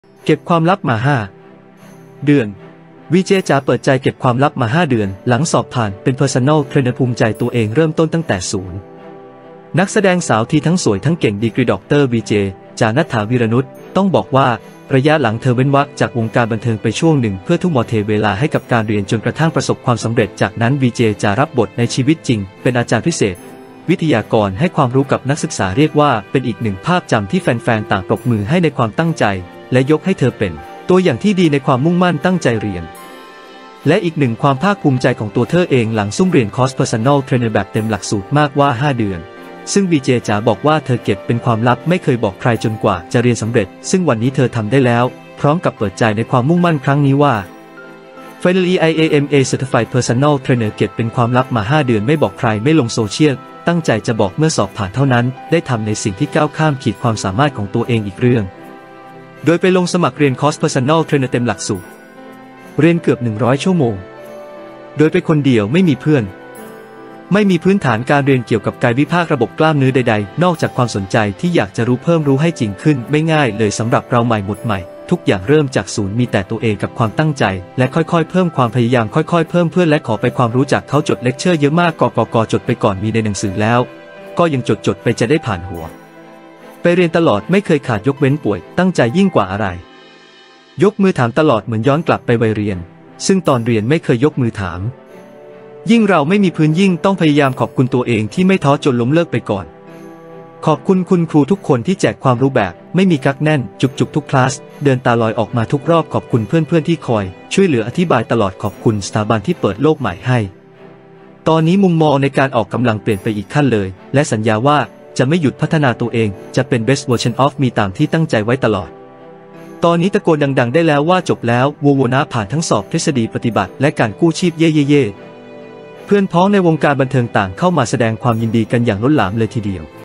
คนร่วมยินดี "วีเจจ๋า" เก็บความลับมา 5 เดือน บอกทั้งน้ำตาวันนี้ทำสำเร็จแล้ว
วีเจจ๋า เปิดใจเก็บความลับมา 5 เดือน หลังสอบผ่านเป็น Personal Trainer ภูมิใจตัวเองเริ่มต้นตั้งแต่ศูนย์